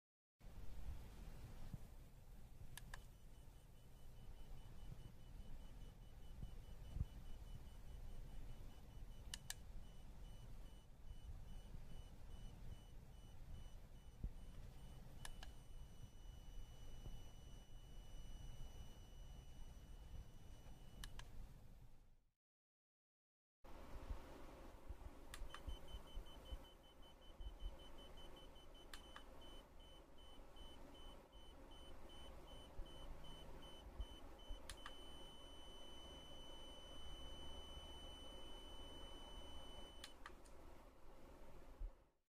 EL wire lighting test
I bought an EL wire in Switch Science.
It's same as Adafruit's one.